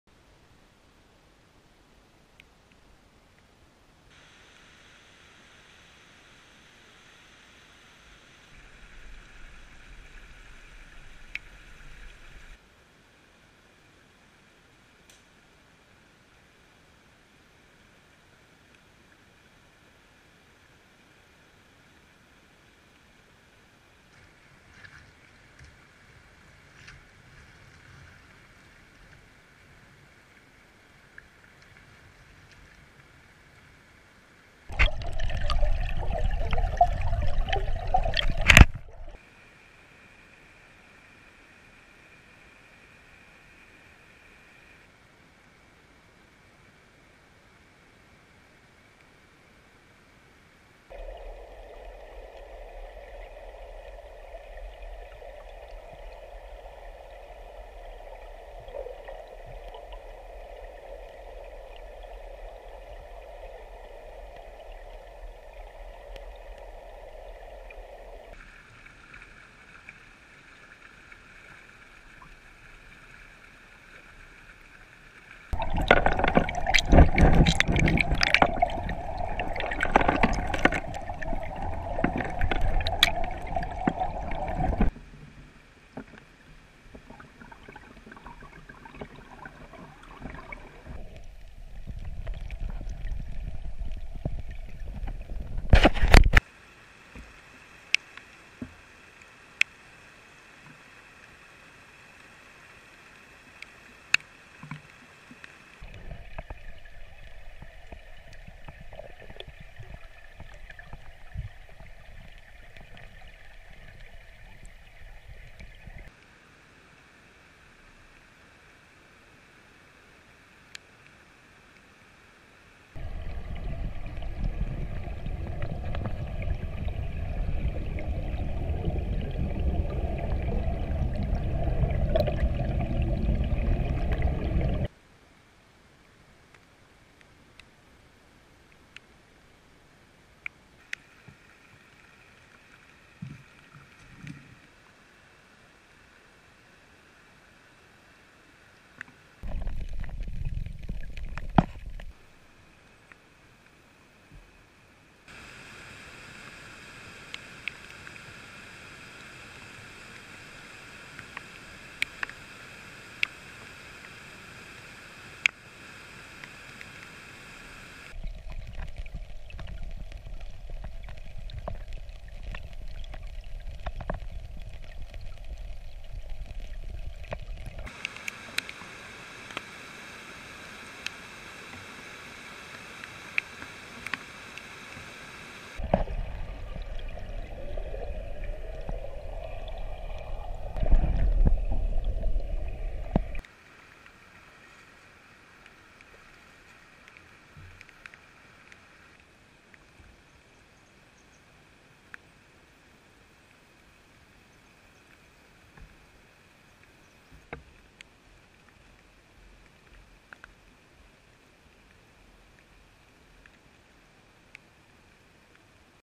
Exploring a Vancouver Island stream.
A swift flowing low nutrient stream with an anadromous barrier within a few kilometres of tide water. Fairly typical of the ECVI. Chum salmon were found in the lower reaches with Coho spread throughout.